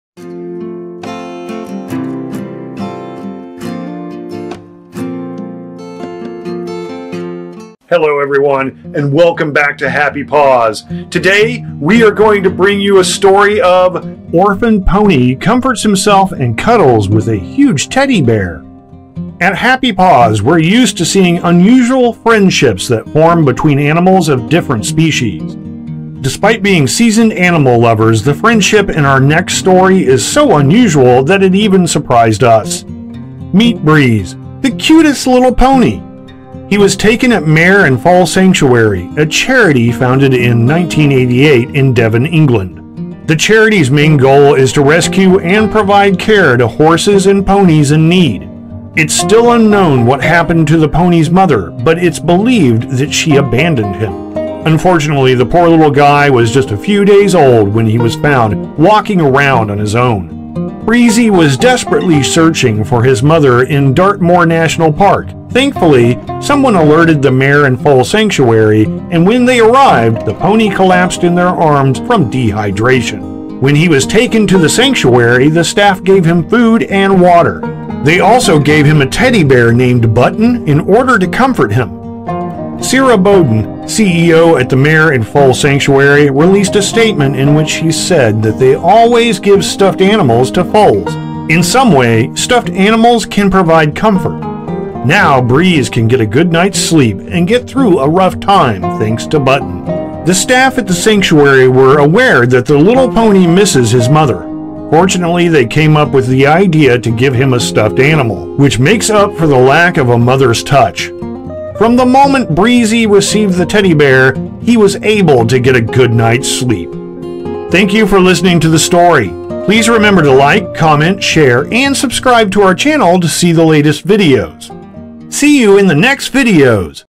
Oprhaned Pony Comforts Himself And Cuddles With A Huge Teddy Bear
At Happy Paws were used to seeing unusual friendships that form between animals of different species. Despite being seasoned animal lovers, the friendship in our next story is so unusual that it even surprised us.

Meet Breeze, the cutest little pony! He was taken at Mare and Foal Sanctuary, a charity founded in 1988 in Devon, England. The charity’s main goal is to rescue and provide care to horses and ponies in need.